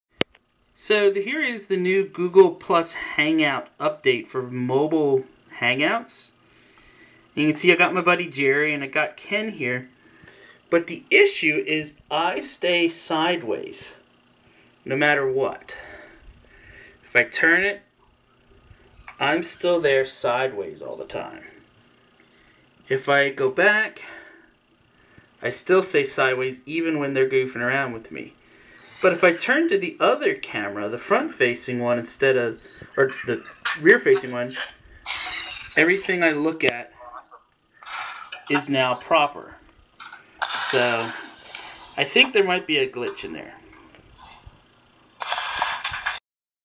Google+ Hangouts Go Mobile- Google+ Mobile Hangouts
Today Google+ hangouts opened a new feature to allow you to do hangouts from your mobile phone.  Unfortunately for my Android device my photo stays sideways the whole time.